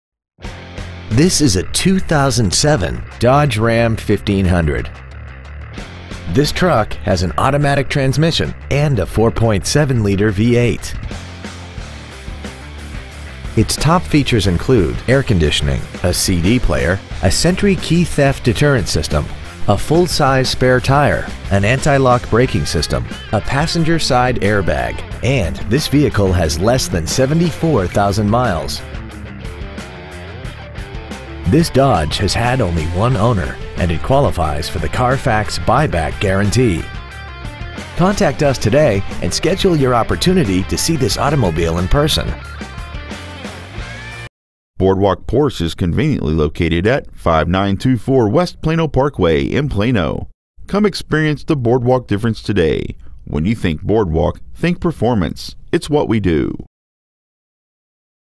2007 Dodge Ram 1500 Plano TX 75093
Year: 2007
 Make: Dodge
 Model: Ram 1500
 Engine: Gas/Ethanol V8 4.7L/287
 Trans.: Automatic
 Exterior: Bright Silver Metallic
 Miles: 73,680
 Interior: Medium Slate Gray
 Stock: PT7980

 2007 Dodge Ram 1500 Plano TX